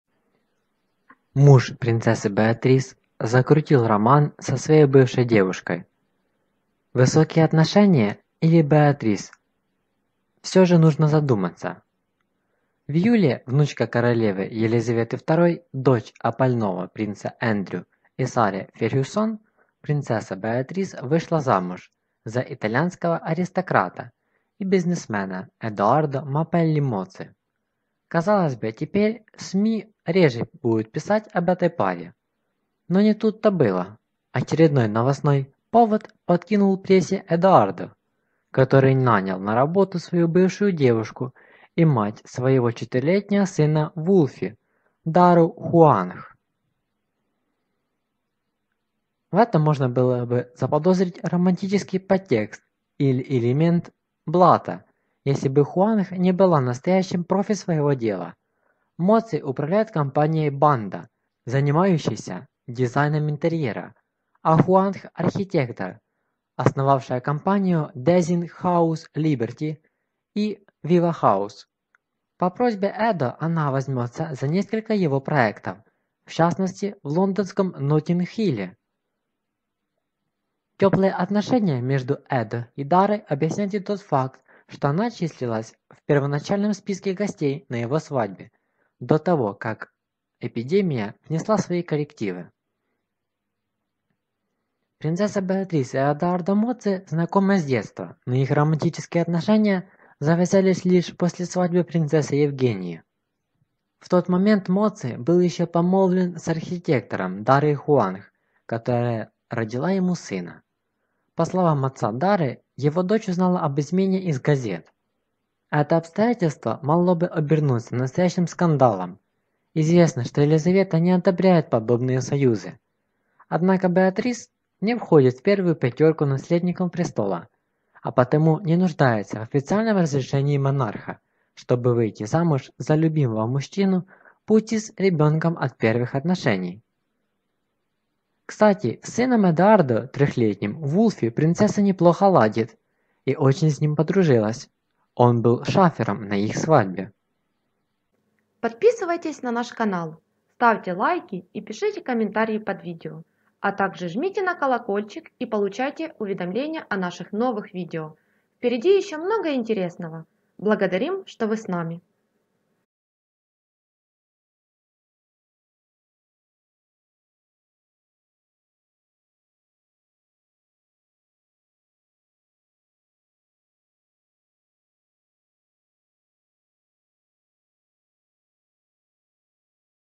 Муж принцессы Беатрис Эдоардо закрутил роман с бывшей девушкой Дарой Хуанг! Шокирующие подробности!
Принц Гарри, Меган Маркл, Кейт Миддлтон, принц Уильям, Елизавета!

В июле внучка королевы Елизаветы II, дочь опального принца Эндрю и Сары Фергюсон - принцесса Беатрис - вышла замуж за итальянского аристократа и бизнесмена Эдоардо Мапелли Моцци. Казалось бы, теперь СМИ будут реже писать об этой паре, но не тут-то было. Очередной новостной повод подкинул прессе Эдоардо, который нанял на работу свою бывшую девушку и мать своего 4-летнего сына Вулфи, Дару Хуанг.

В этом можно было бы заподозрить романтический подтекст или элемент блата, если бы Хуанг не была настоящим профи своего дела. Моцци управляет компанией Banda, занимающейся девелопментом и дизайном интерьера, а Хуанг - архитектор, основавшая компании Design Haus Liberty и Vivahouse. По просьбе Эдо она возьмется за несколько его проектов - в частности, в лондонском Ноттинг-Хилле. Теплые отношения между Эдо и Дарой объясняет и тот факт, что она числилась в первоначальном списке гостей на его свадьбе - до того, как эпидемия внесла свои коррективы.

Принцесса Беатрис и Эдоардо Моцци знакомы с детства, но их романтические отношения завязались лишь после свадьбы принцессы Евгении. В тот момент Моцци был еще помолвлен с архитектором Дарой Хуанг, которая родила ему сына. По словам отца Дары, его дочь узнала об измене из газет. Это обстоятельство могло бы обернуться настоящим скандалом - известно, что Елизавета не одобряет подобные союзы. Однако Беатрис не входит в первую пятерку наследников престола, а потому не нуждается в официальном разрешении монарха, чтобы выйти замуж за любимого мужчину, пусть и с ребенком от прошлых отношений. Кстати, с сыном Эдоардо - 3-летним Вулфи - принцесса неплохо ладит.